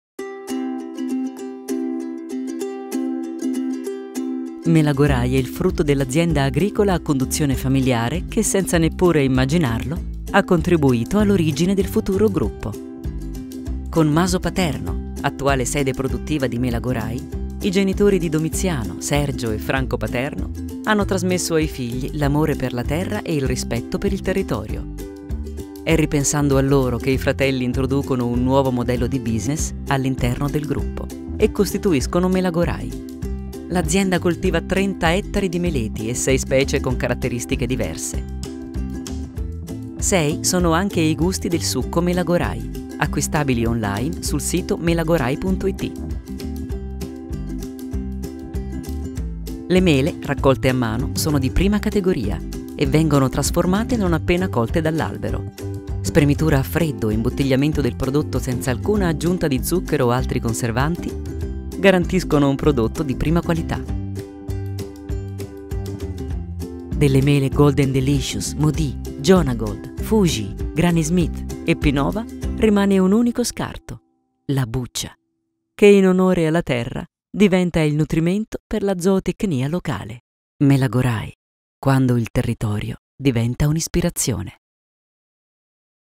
Gruppo Paterno: Melagorai
Melagorai è il succo di mela prodotto in sei gusti dall'azienda agricola Maso Paterno, alle pendici della catena montuosa del Lagorai.
Succo Melagorai: 100% natura, 0 zuccheri aggiunti.